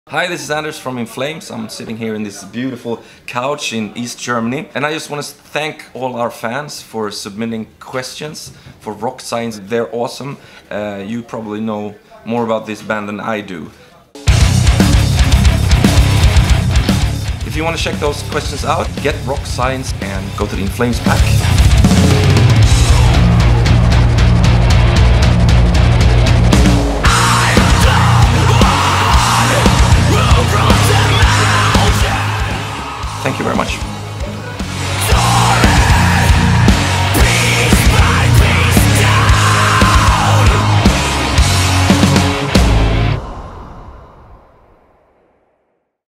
A message from Anders Friden of In Flames
The In Flames Pack with trivia questions is available now in the free game Rock Science Universe!